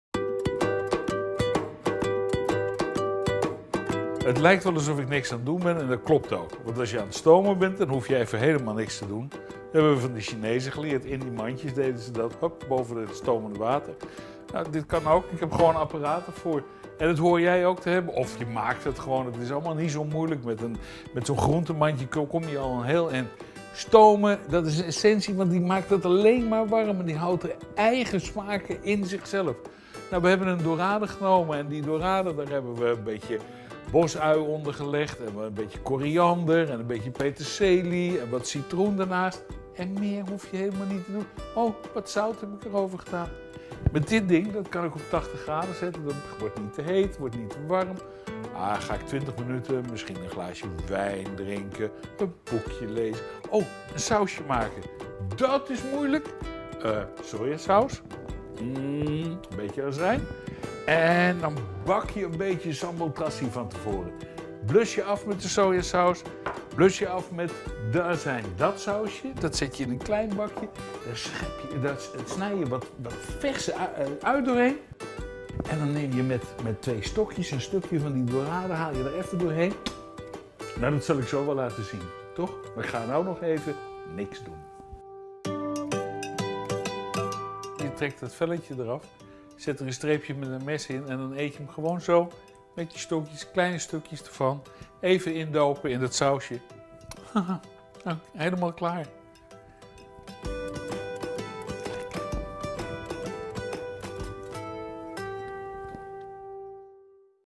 'Stomen is de essentie'
In deze aflevering van Felix met Smaak vertelt culinair journalist Felix Wilbrink over het stomen van je eten. Ook op deze manier krijg je heerlijke gerechten op tafel!